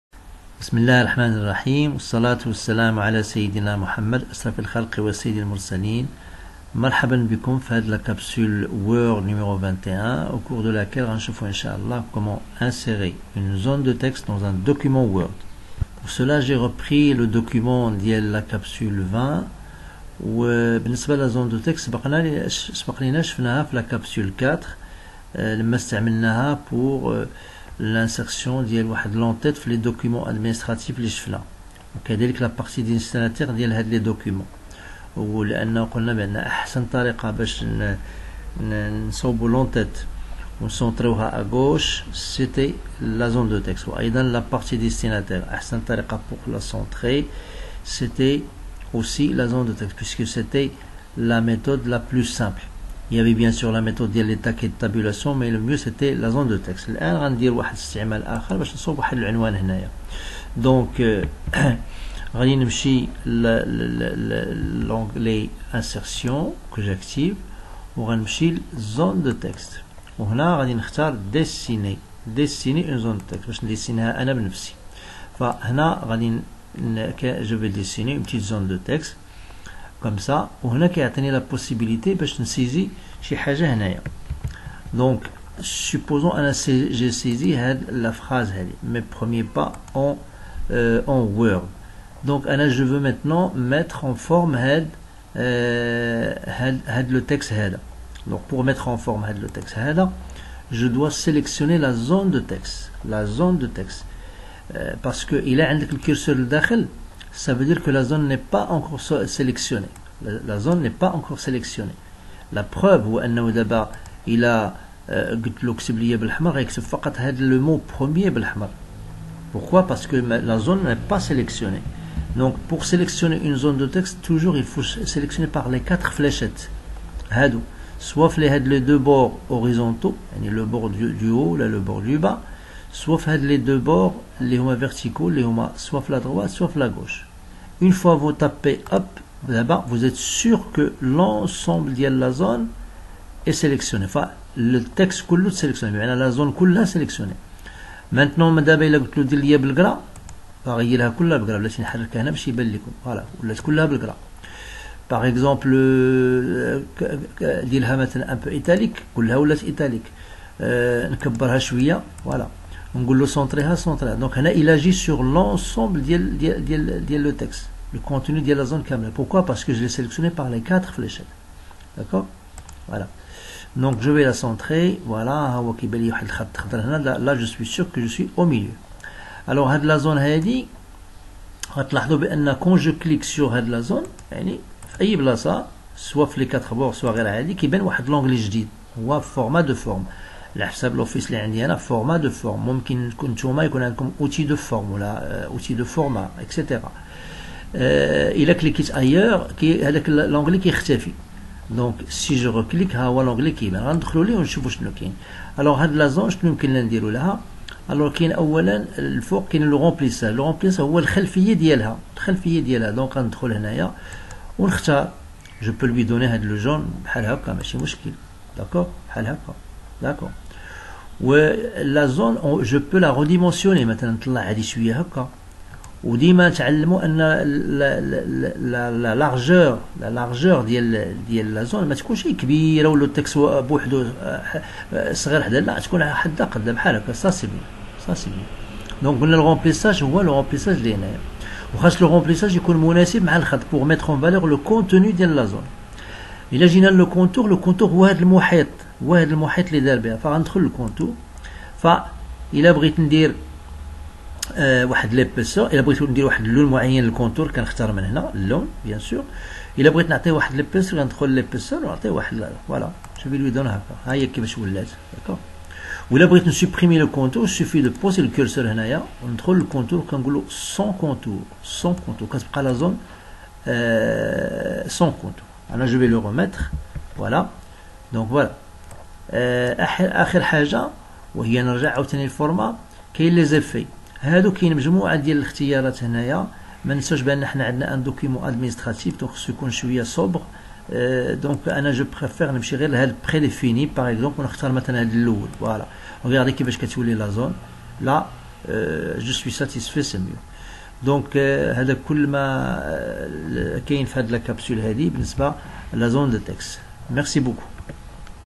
Capsule Word N°21 - Insertion d’une zone de texte
السلام عليكم (ن) ورحمة الله تعالى وبركاته.
هذه الكبسولة ضمن سلسلة من الكبسولات التكوينية الهادفة في برنامج وورد والموجهة لموظفي وموظفات الجماعات الترابية
Cette capsule est dans le cadre d’une série de capsules de formation Word, ciblées et destinées aux fonctionnaires des Collectivités Territoriales

Pour une meilleure visualisation de la capsule :
- Cliquez en bas à droite de la fenêtre Youtube, sur la roue crantée
- Dans le menu qui se déploie, cliquez sur Qualité.
- Choisissez la qualité supérieure.